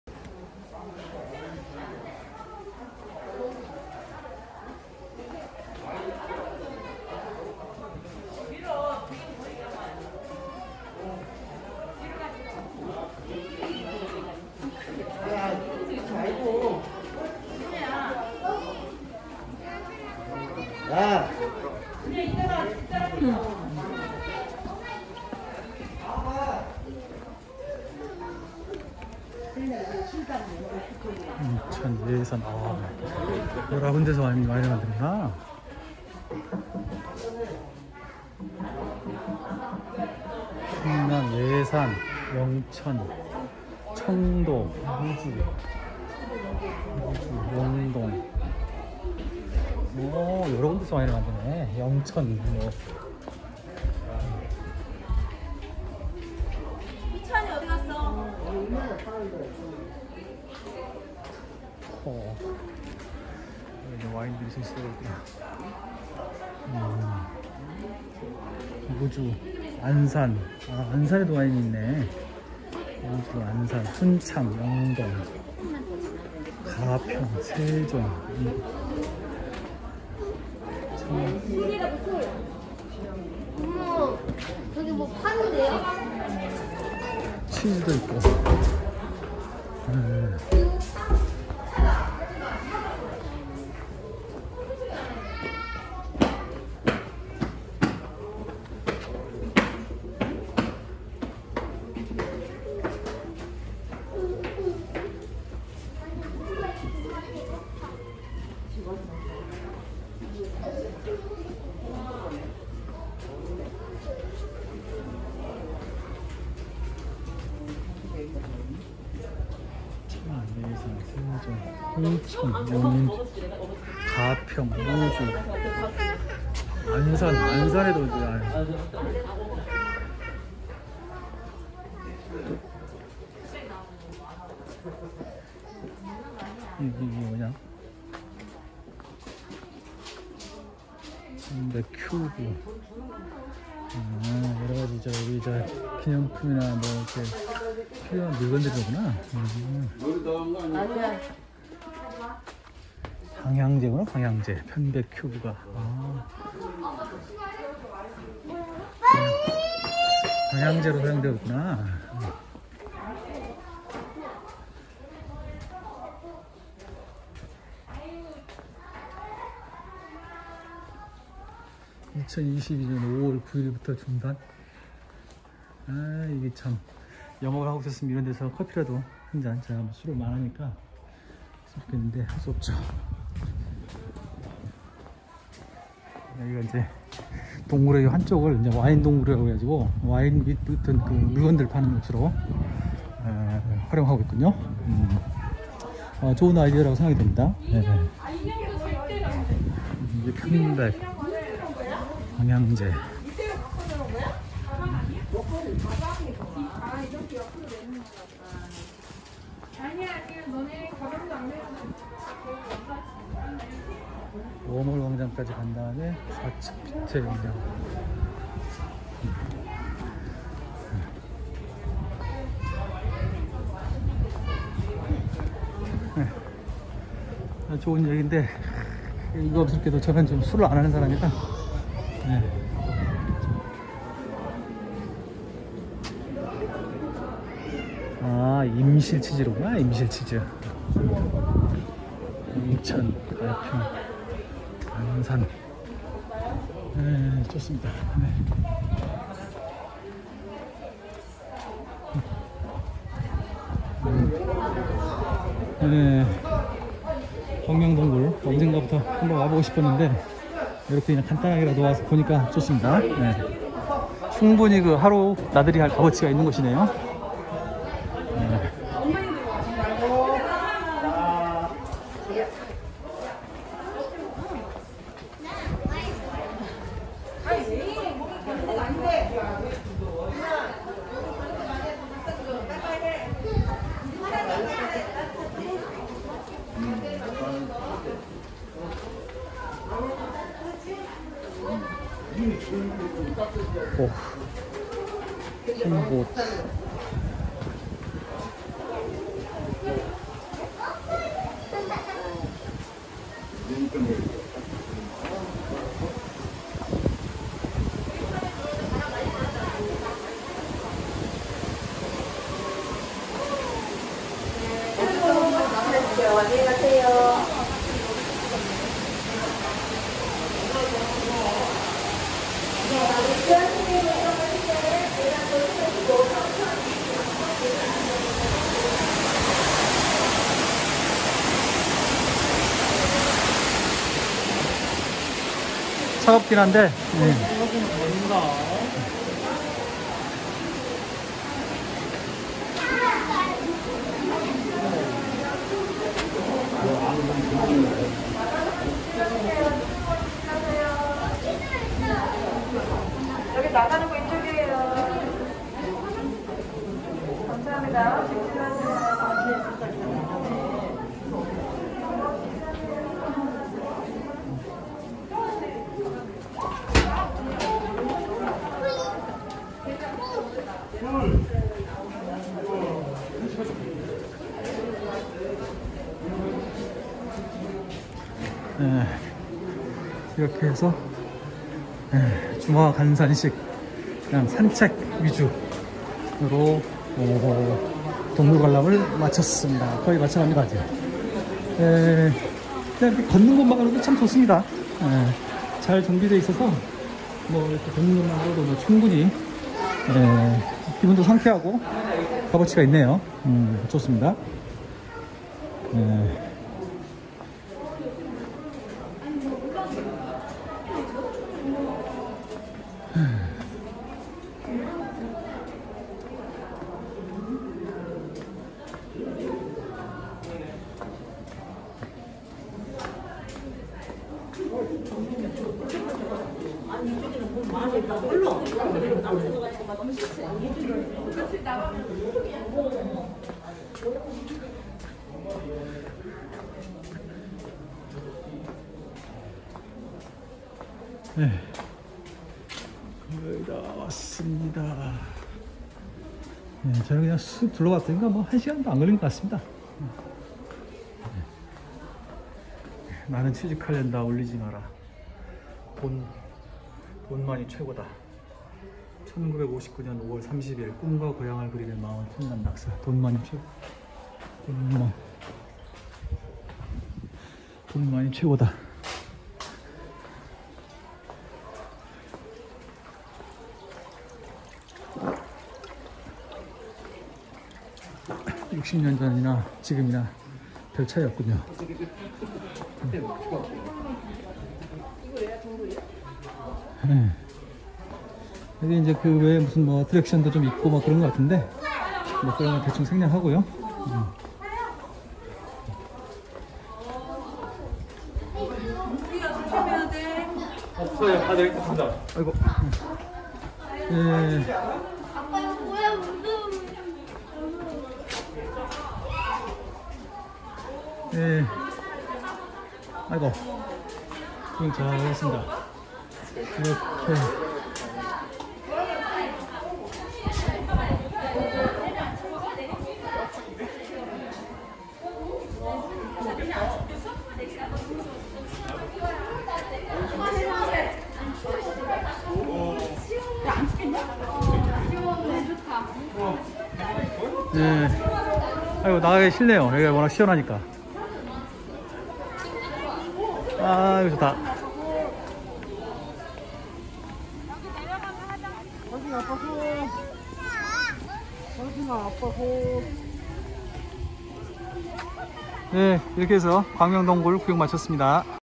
(9)(2022-광명) 와인동굴 구경, 광명동굴 탐방 종료
와인 부분 구경을 마지막으로 동굴 구경을 마쳤습니다. 국내 100여 종의 와인을 비치하고 판매하고 있다고 합니다. 국내 와인은 사과 등 포도 아닌 과일을 이용한 와인까지 포함된다고 하는데, 안산에서도 와인이 난다는 것을 처음 알게 되었습니다. 임실치즈도 판매하고 있었습니다.
맨 끝에는 레스토랑이 있었으나, 5월에 문을 닫았다고 합니다.
이것으로 동굴 탐방을 마치고 밖으로 나왔습니다.